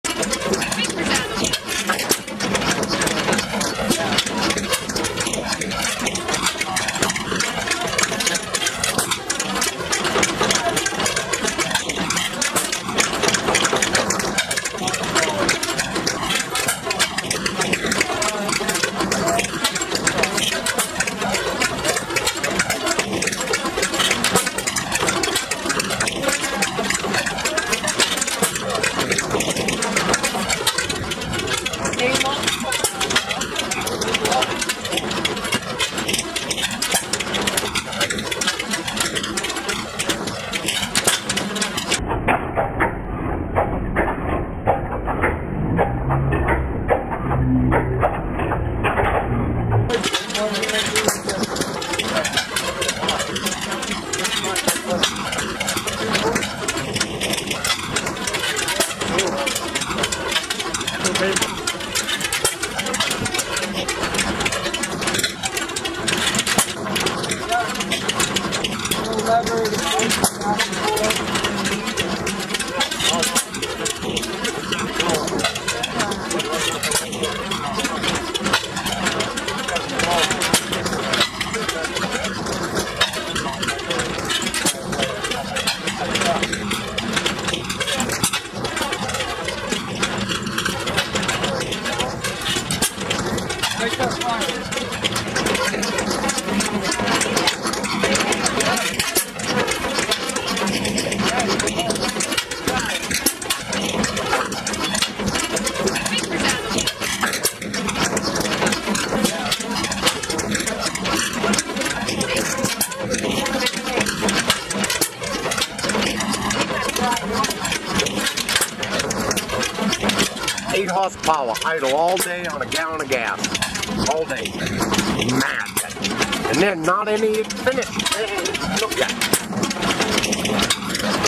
HOMEMADE LOG Splitter 1890 Fire wood splitting
1890s wooden log splitter home made on the farm. Powered by a 8hp hit and miss engine, burns one gallon of gasoline in 12 hours. A 2 man wood splitter for splitting firewood by the cord. Double wedge splitter having 2 strokes per second. Fly wheel log splitter. Faster than it looks at splitting wood.